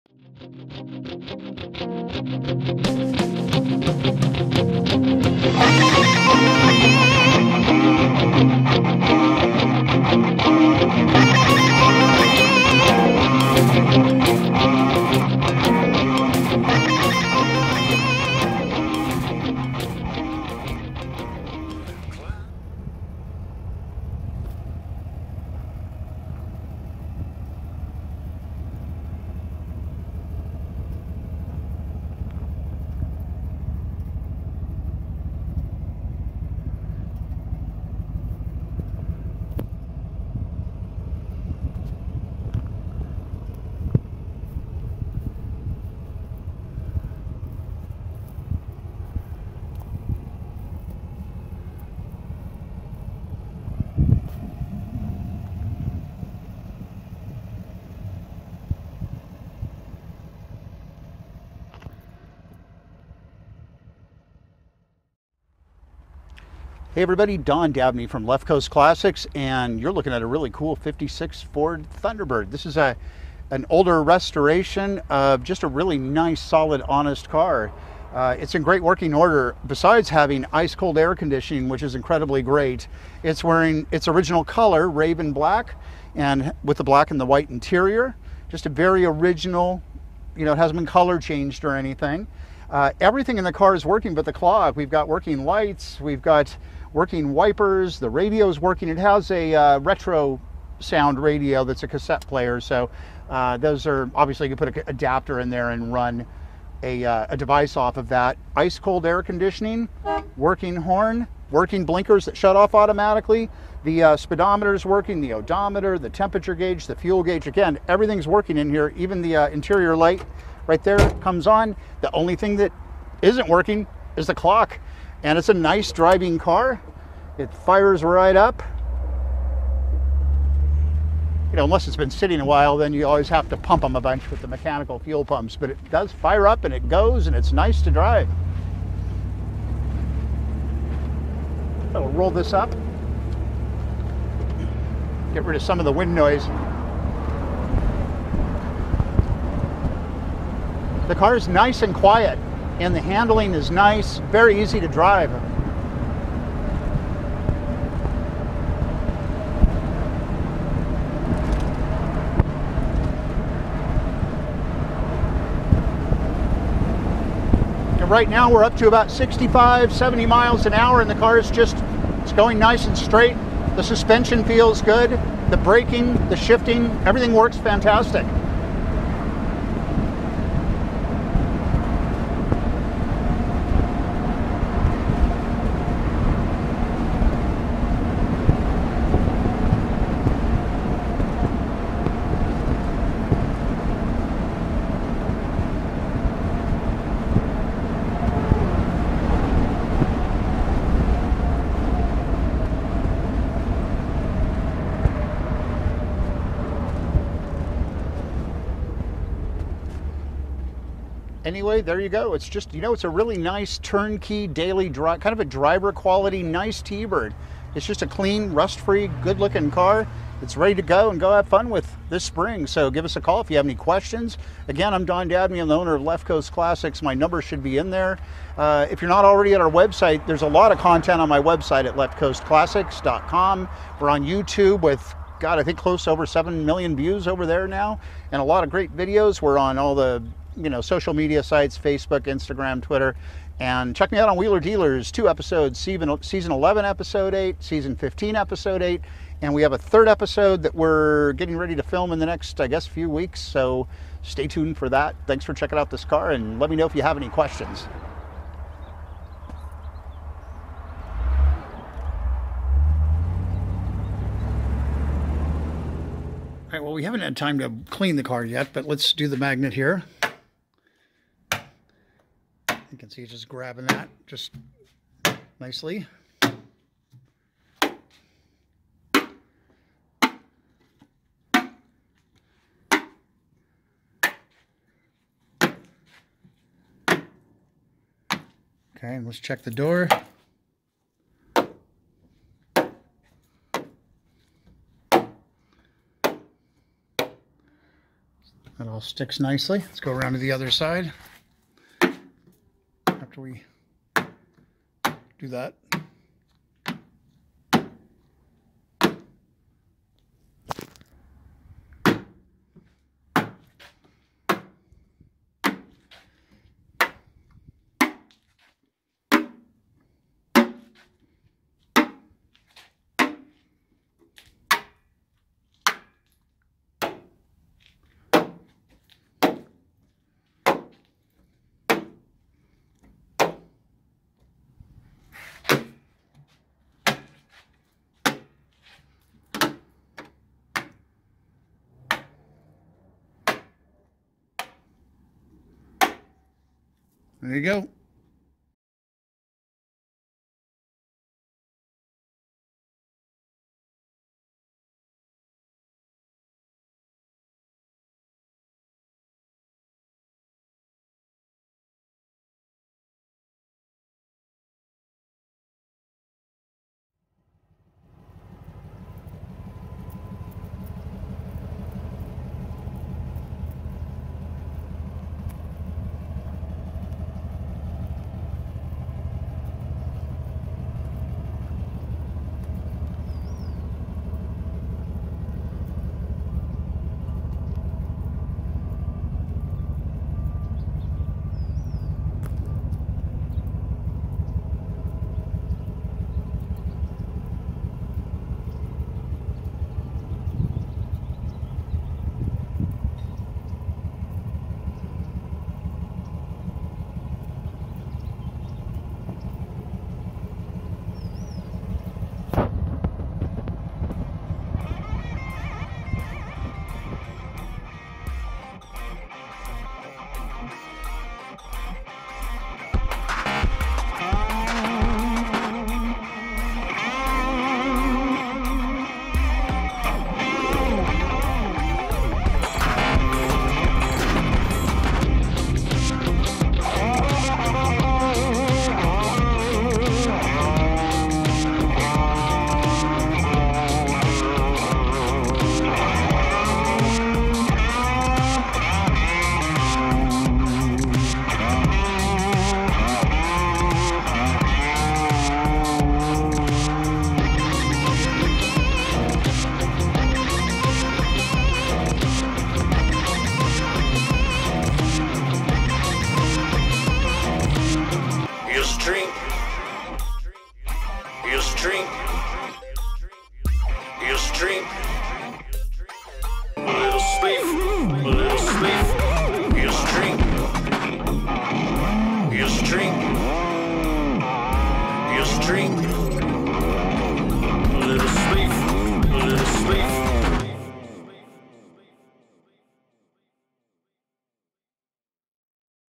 1956 Ford Thunderbird in Sonoma California
to see this 1956 Ford Thunderbird sold by Left Coast Classics in Sonoma California.

This is a special Thunderbird ready for a new home and many more adventures.

This lovely 1956 Ford Thunderbird for sale was sold new in California, later living in Arizona for many years before returning to California again in 2015. It was built in June of 1956 for the San Jose, California sales office and is finished in all of its original colors, Raven Black with black and white interior and a black canvas top.

The 312 cubic inch V8 starts and runs very nicely with plenty of power. The Ford-o-Matic shifts smoothly through all of the gears and the brakes, steering and suspension feel good. All in all, it is a very nice driving car that seems to have been nicely cared for over the years. Due to the age of this Thunderbird expect some damp seals.

It is equipped with the 312ci Fordomatic 225HP engine, power steering, both hard and soft tops, engine dress-up kit and an aftermarket ice-cold air conditioning unit.

The body is straight and rust-free with good panel fitment all around. The older Raven black paint looks nice and presentable with a nice shine. The top, trim, chrome and glass look great as well. It has a newer, very nice cloth top. The original undercarriage is exceptionally sound and rust-free as well. Overall this is a very nice looking Thunderbird.

All of the gauges and accessories are in working order except for the clock. The modern aftermarket air conditioning blows ice cold. It is a very comfortable car for long drives or around town.

The inside of the car is quite clean and comfortable with nice seating and carpets. It looks to still have its original door panels and headliner on the hardtop.

If you would like more information about this 1956 Ford Thunderbird take a look for the full detailed description and to see the photo gallery of this cool Thunderbird.

Donn Dabney